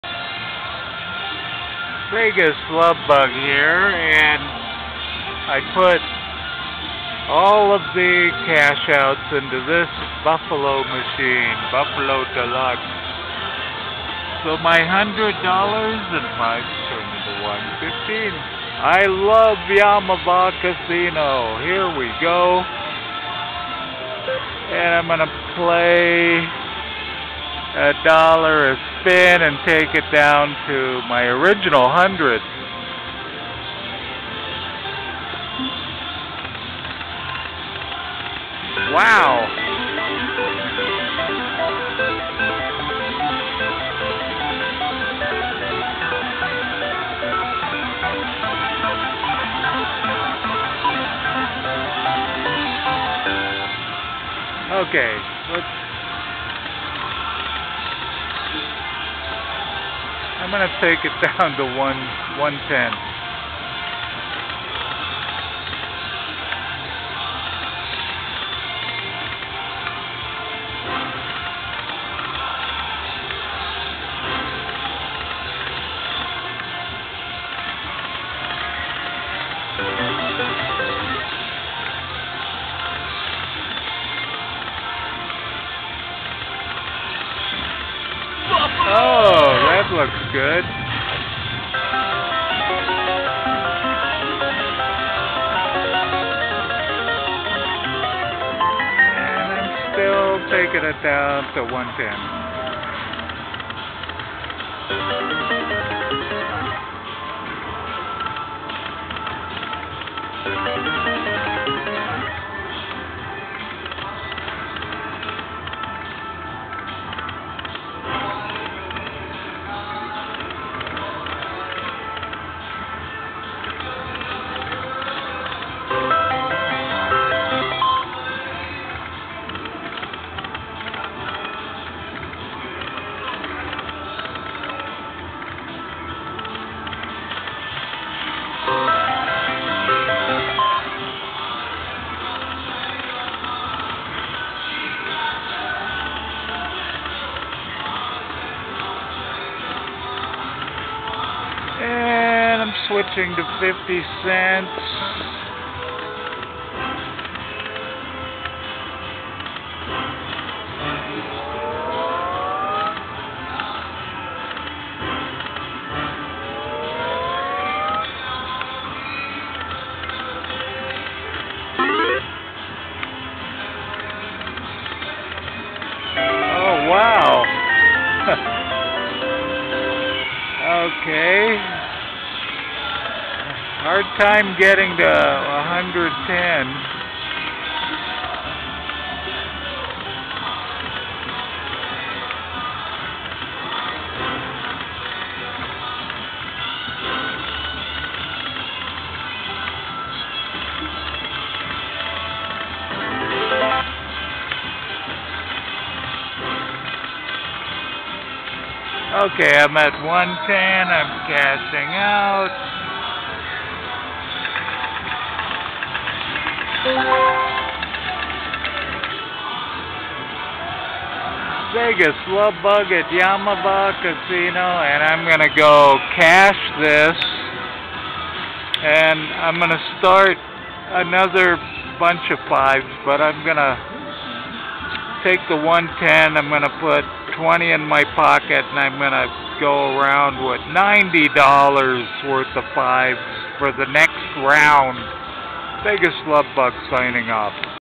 #6 CASHING CASH OUTS IN A BUFFALO MACHINE & PLAYED A BIT MORE TO OVER $100.00, NICE! VegasLoveBug
Yes!  It was a pretty good day at Yaamava' casino!  VegasLoveBug